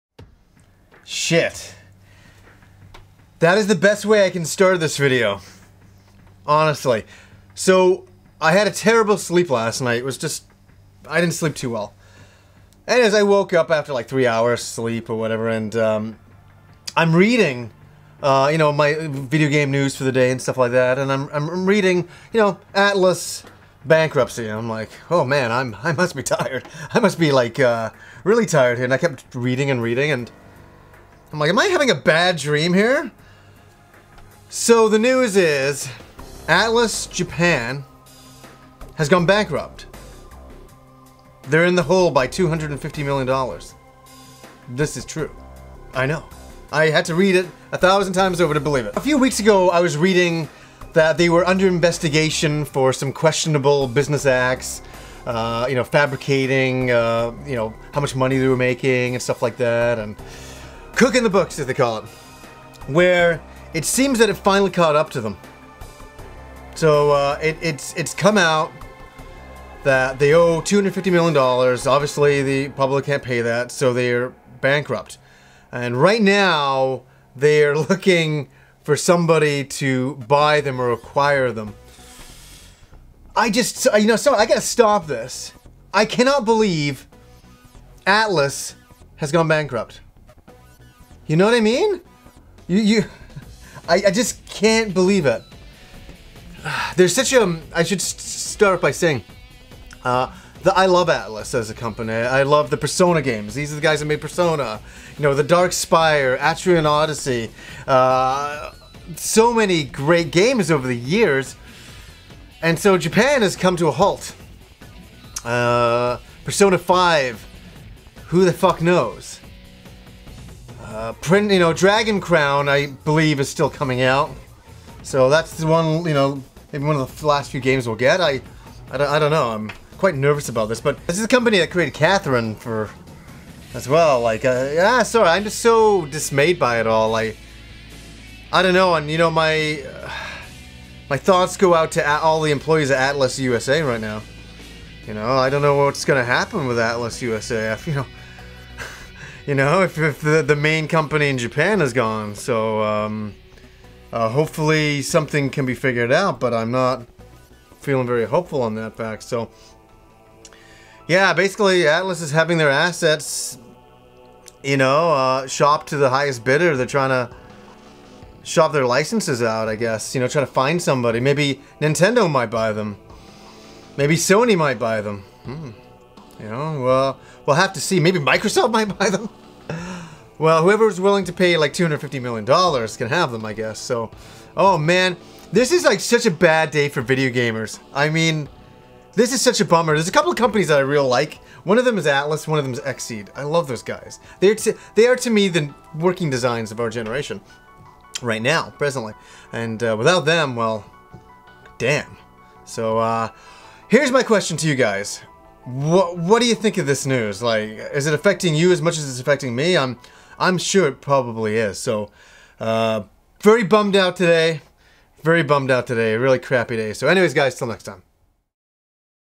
ATLUS GOES BANKRUPT!?? - Happy Console Gamer
Index Corporation Japan files for Bankruptcy which affects Atlus..